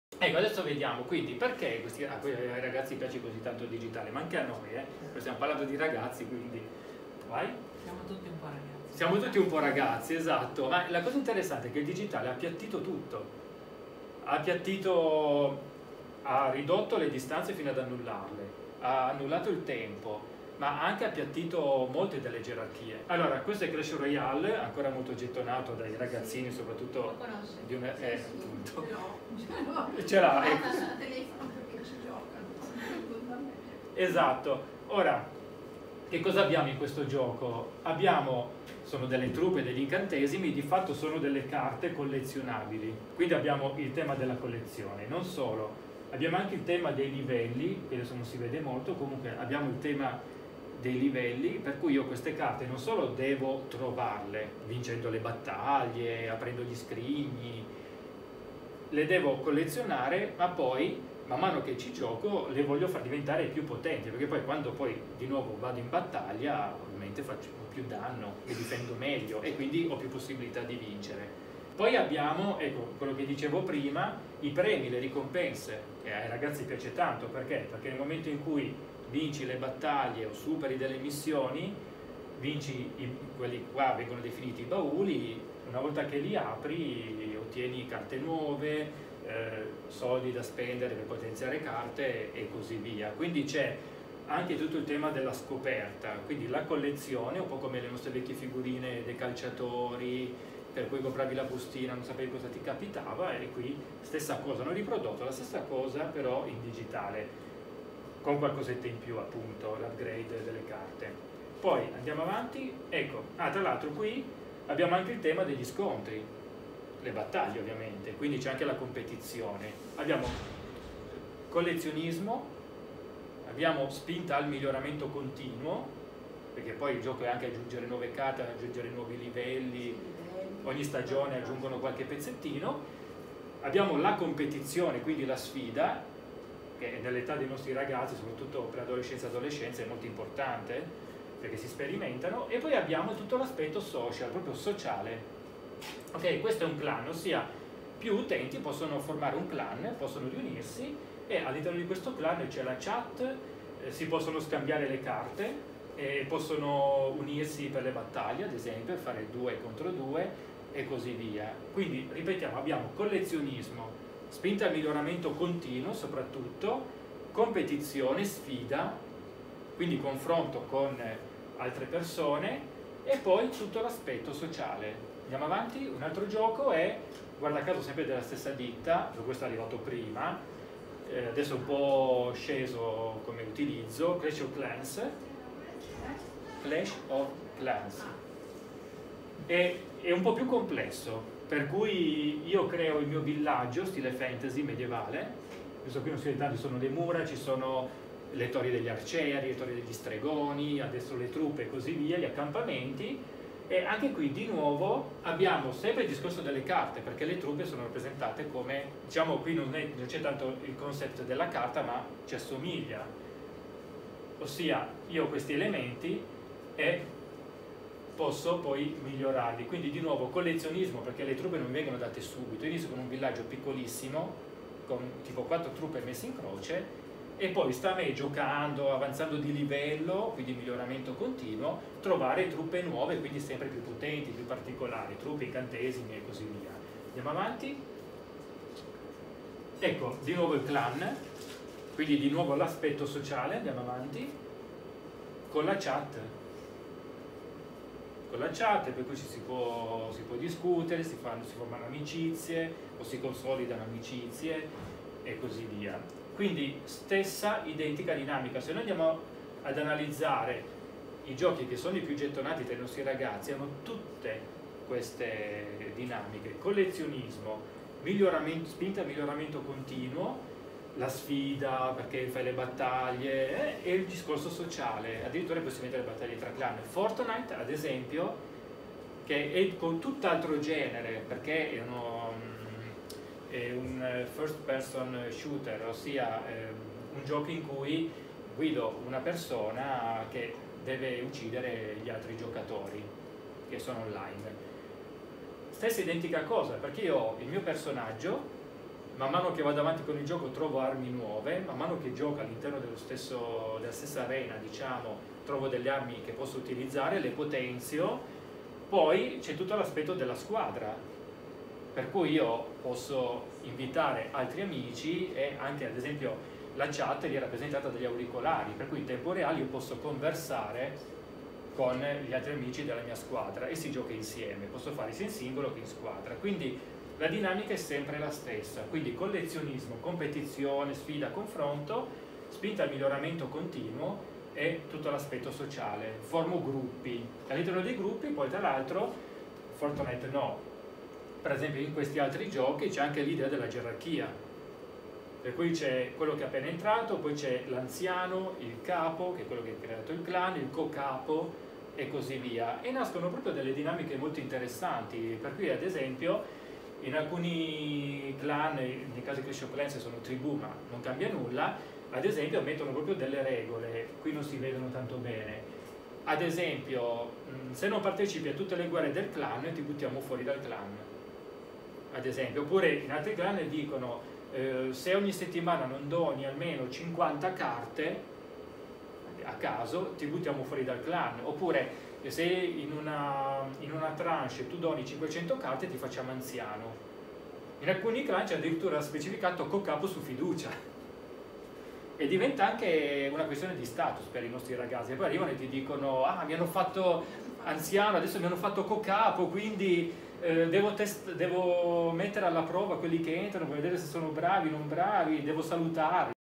La UX Design dei videogiochi preferiti dai nostri ragazzi | Fortnite Clash of Clans Clash Royale
Fortnite, Clash of Clans, Clash Royale, che cosa hanno in comune?
Perchè piacciono così tanto ai nostri ragazzi?
In tutti questi anni ho analizzato molti giochi dal punto di vista della UX Design, e ho scoperto che tutti i videogames che appassionano i nostri ragazzi hanno degli elementi in comune:
- il collezionismo
- la suspence della scoperta
- il potenziamento continuo
- la sfida (challenge)
- il sociale
Vi presento un altro video tratto dall’evento Social Corner organizzato da Spazio Aperto Servizi a Milano.